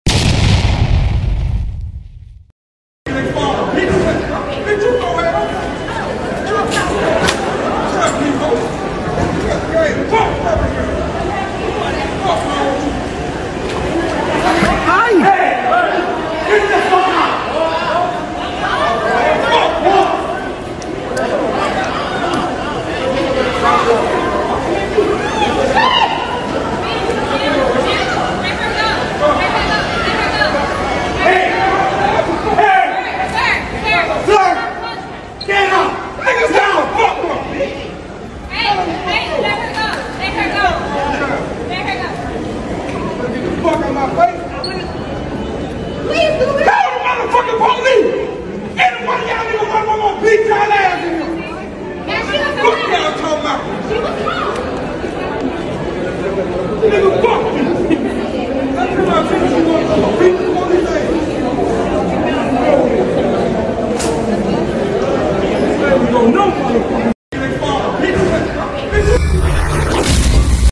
Unbelievable! A Guy Knocked Down A Lady In Wheelchair!!
Watch how in this Incredible video a guy who is three times the size of a lady knocked her down from her wheelchair at the airport! airport puertorico wheelchair thisisbad bad moluscotv we thank Molusco Tv for this footage